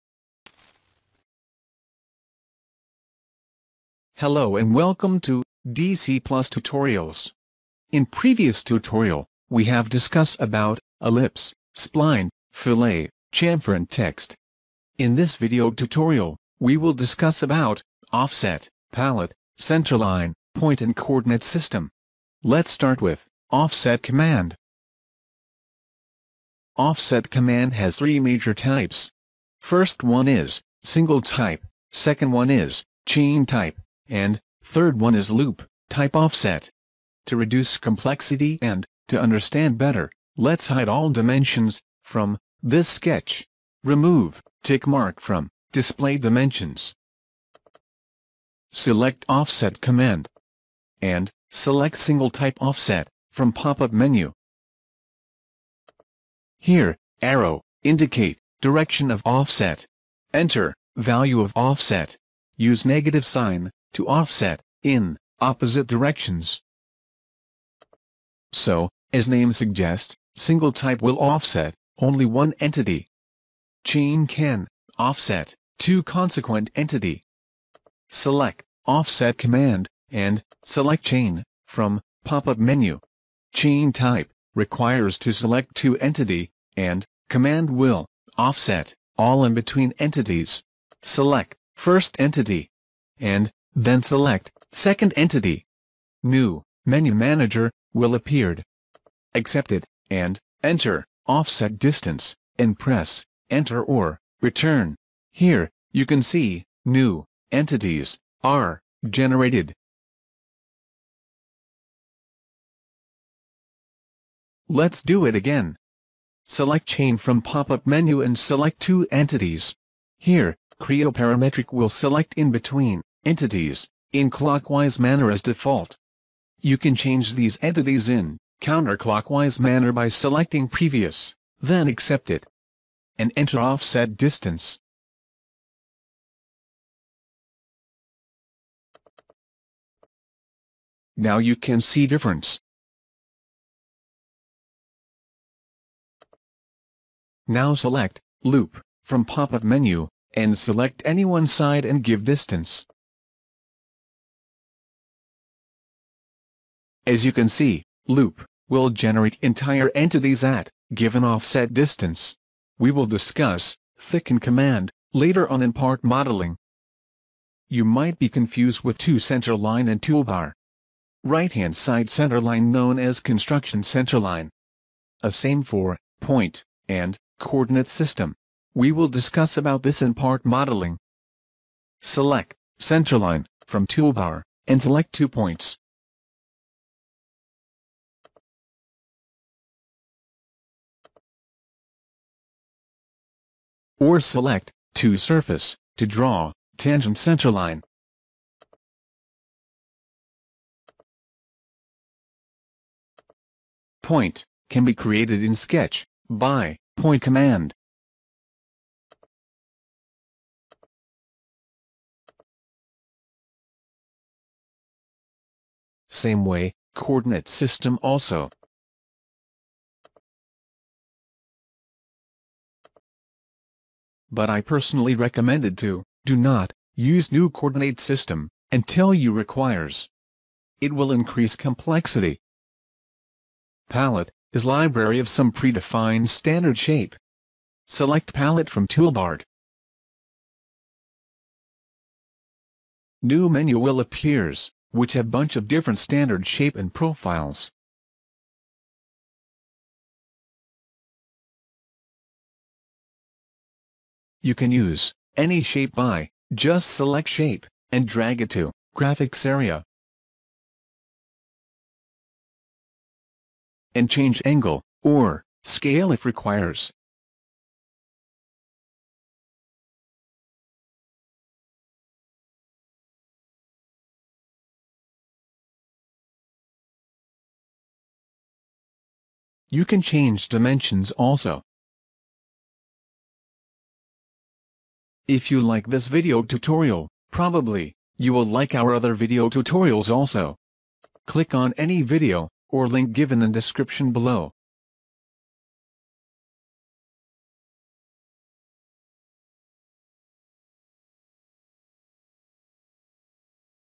PTC Creo Parametric 2.0 Training -Sketch Part 3 - DC+ Tutorials (Basic + Advanced Training)
Welcome to DC+ Tutorials,

This tutorial will explain different command in sketch in PTC Creo Parametric (2.0/3.0). Tutorial for sketch is divided in  parts
First Part includes,
1)  Line
2)  Rectangle
3)  Circle and

In Second Part
5)  Ellipse
6)  Spline
7)  Fillet
8)  Chamfer

10) Offset
11) Thicken
12) Palette
13) Centerline
14) Point
15) Coordinate System

Video includes these how to use these commands.